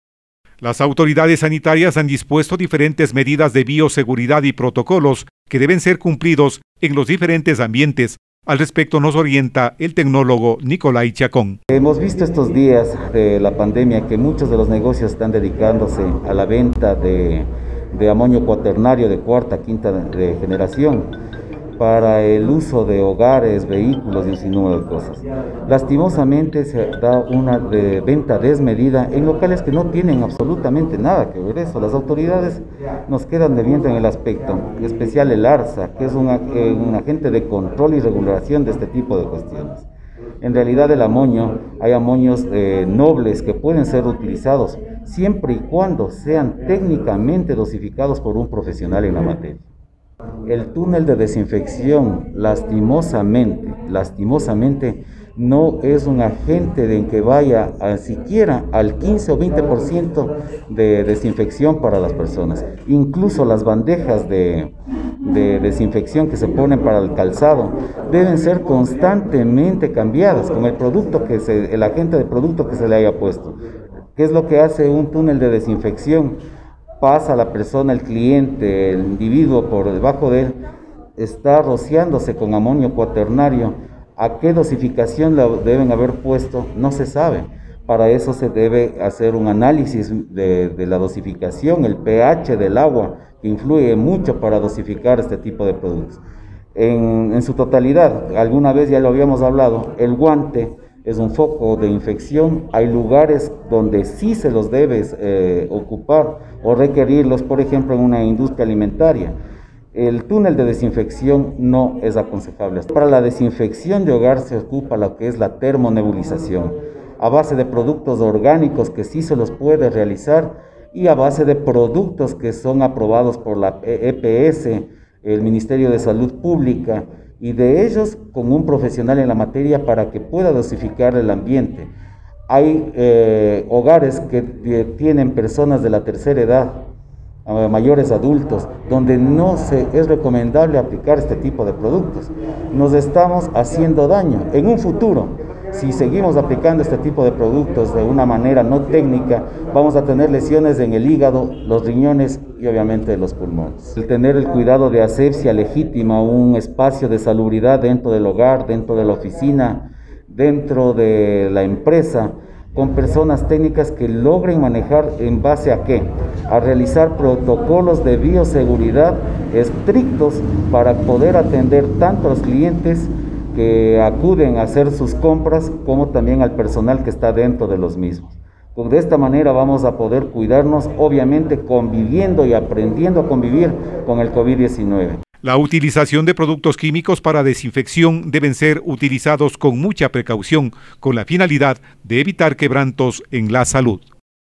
NICOLAI CHACÓN, VIERNES 05 DE JUNIO
Sanitización, o termo-nebulización de ambientes para prevenir contagios por Covid19EC, debe realizarlo personal calidicado, afirma NicolaiChacón.